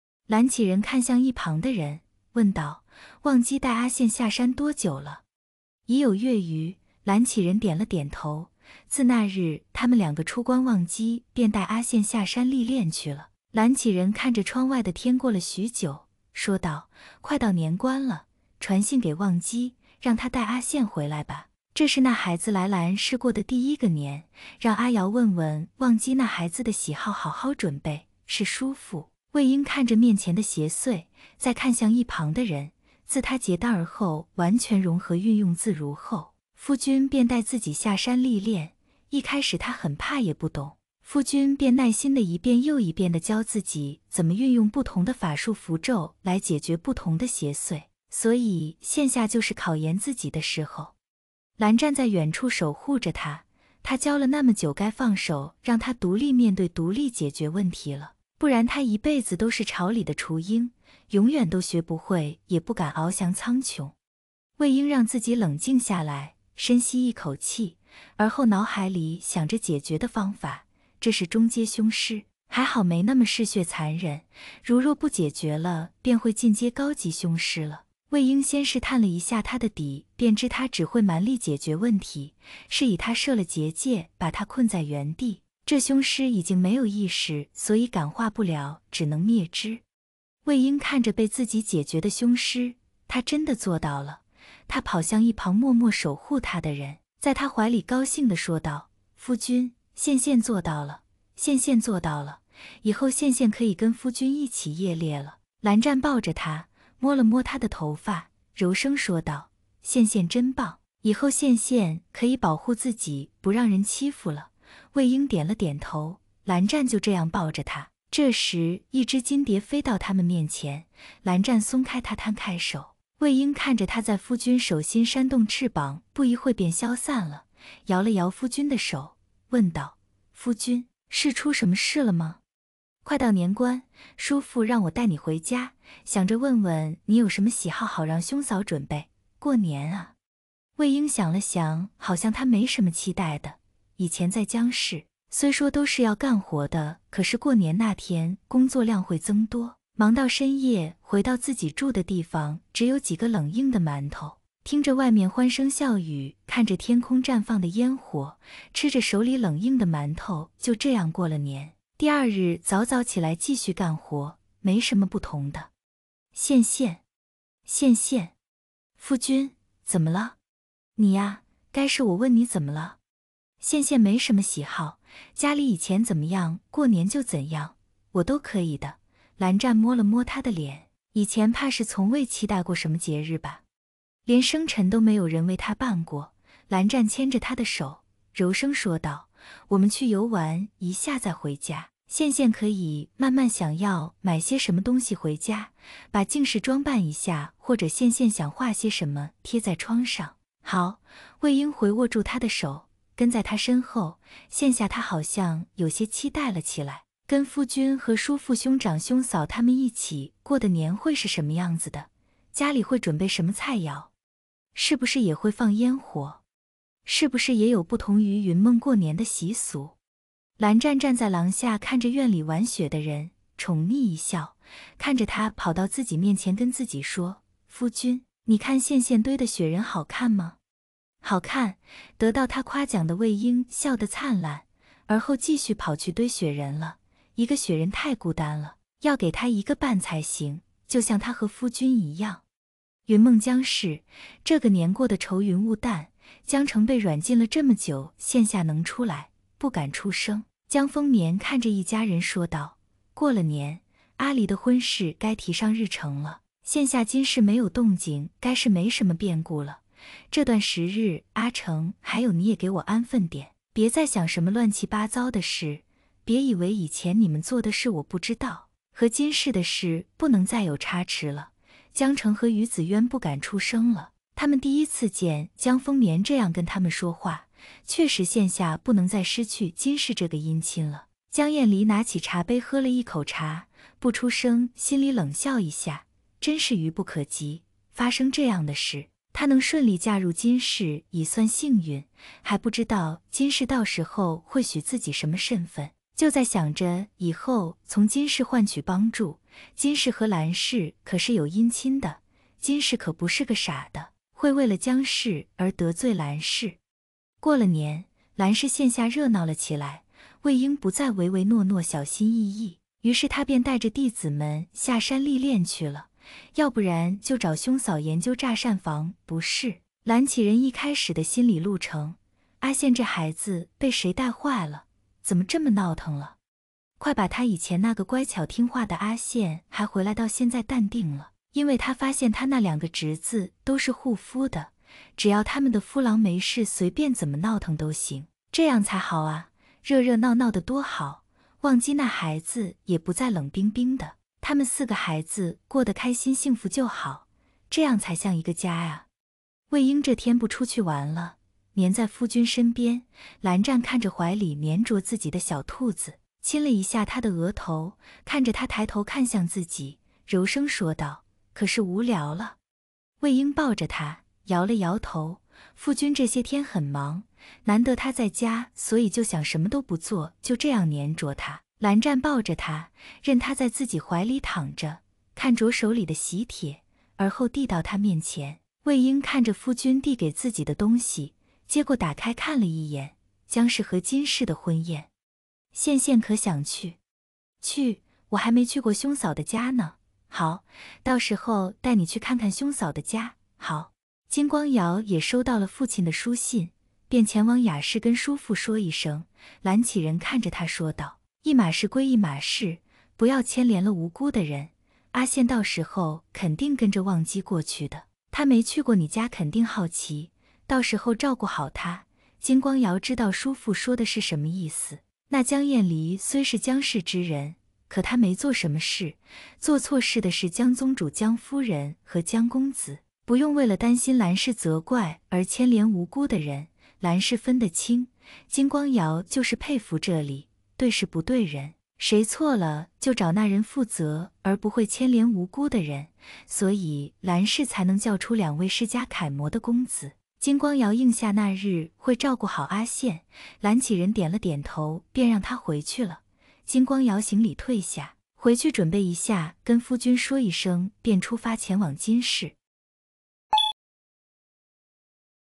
【忘羨】白撿一媳婦07：過了年，藍氏現下熱鬧了起來，魏嬰不再唯唯諾諾小心翼翼，於是他便帶著弟子們下山歷練去了，要不然就找兄嫂研究炸膳房（不是）
藍啟仁一開始的心裏路程，阿羨這孩子被誰帶壞了，怎麽這麽鬧騰了？